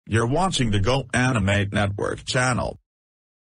Go!Animate Network Bumper - You're Watching the Go!Animate Channel (read desc.)
NOTE: I'm not starting a new network, I'm just making a fanmade logo about a REAL, but fanmade network that I didn't make. It's called "Go!Animate Network". I saw some sign on's and sign off's, and it inspired me to make my own G!AN logo and I'll make more. (I'm still sticking with Random Network)

Go!Animate Logo and "Network" font belongs to Vyond

Go!Animate logo is owned by Vyond

"How did you get the Eric voice!?! I want it!!!"

NOTE: Give me credit, before using my videos.